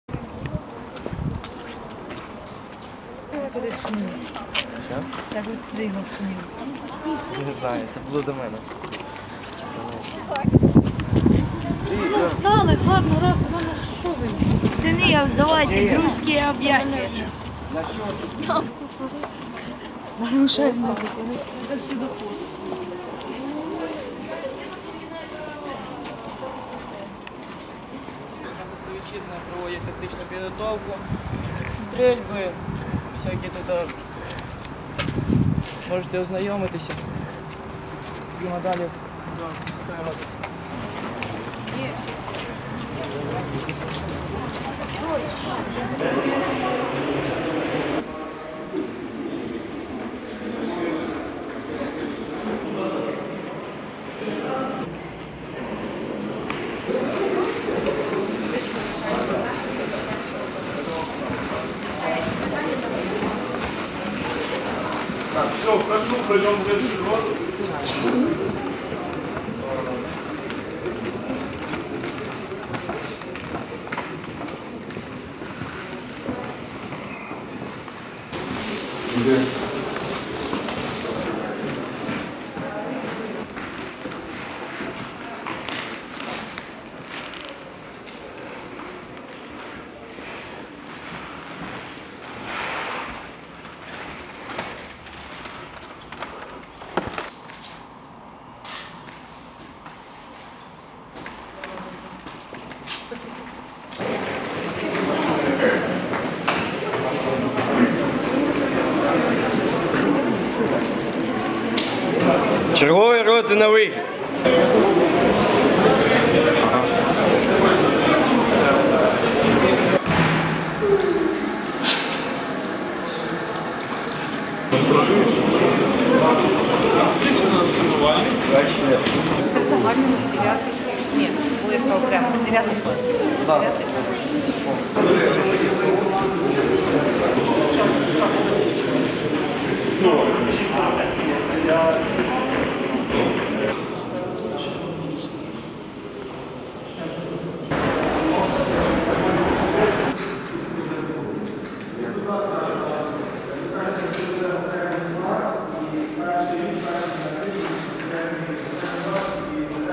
День відкритих дверей в Київському ліцеї ім. Івана Богуна
У Київському військовому ліцеї імені Івана Богуна провели День відкритих дверей.